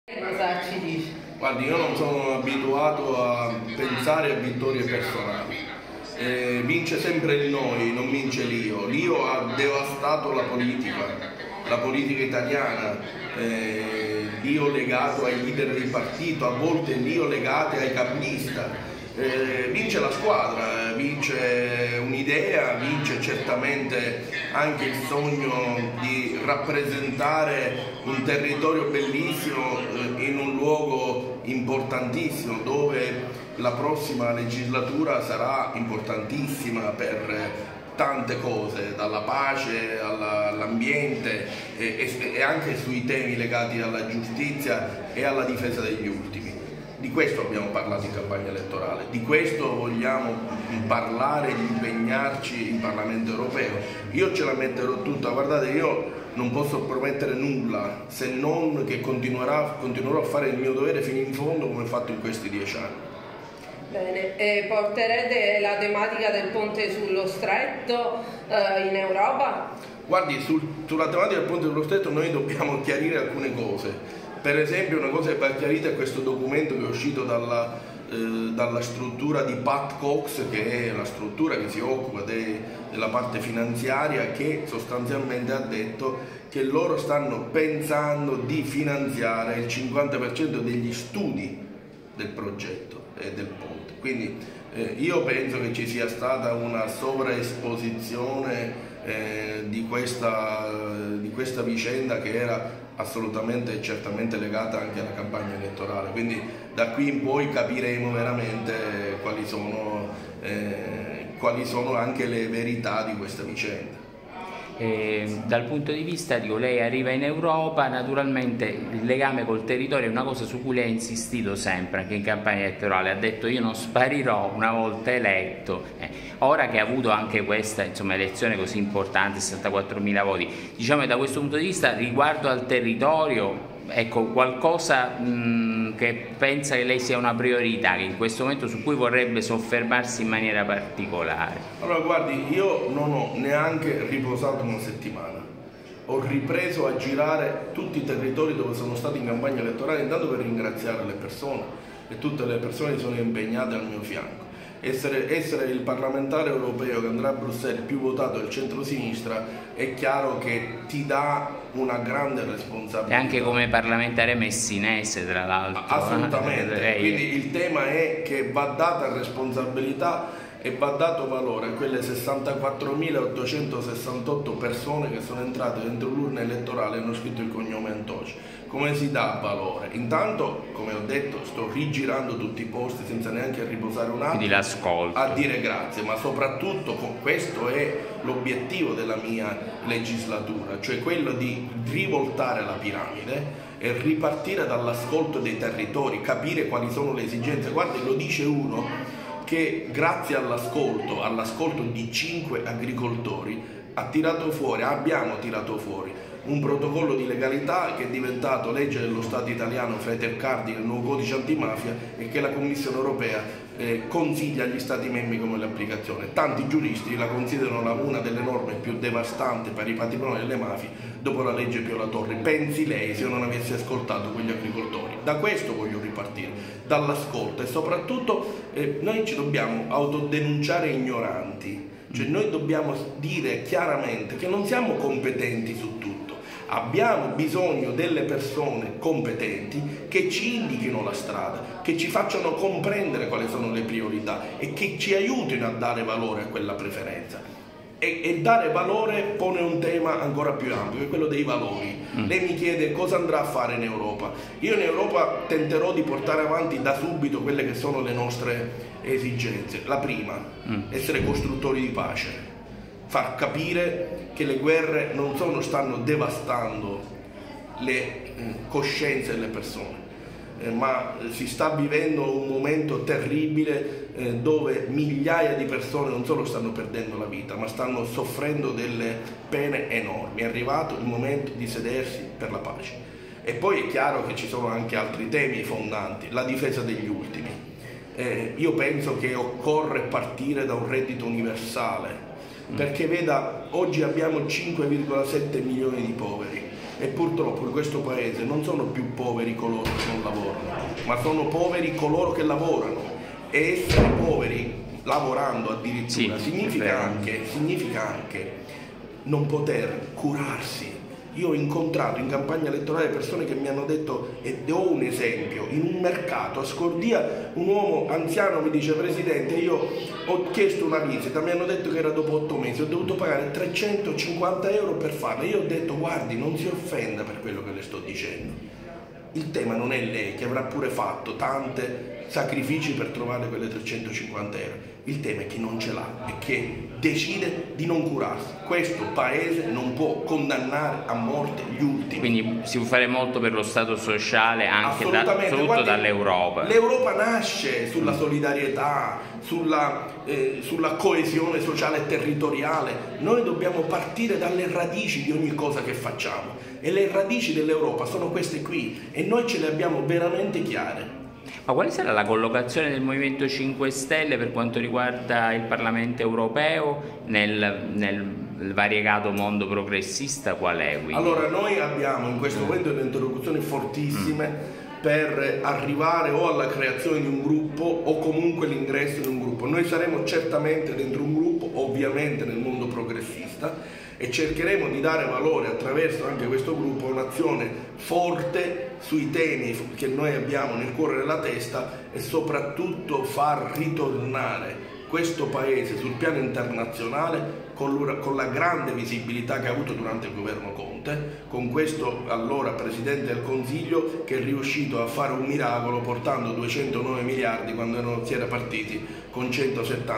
“In Parlamento europeo per dare voce al territorio messinese”
Il neo eletto Giuseppe Antoci ha incontrato i sostenitori del M5S. Unico parlamentare messinese e il più votato nel centrosinistra.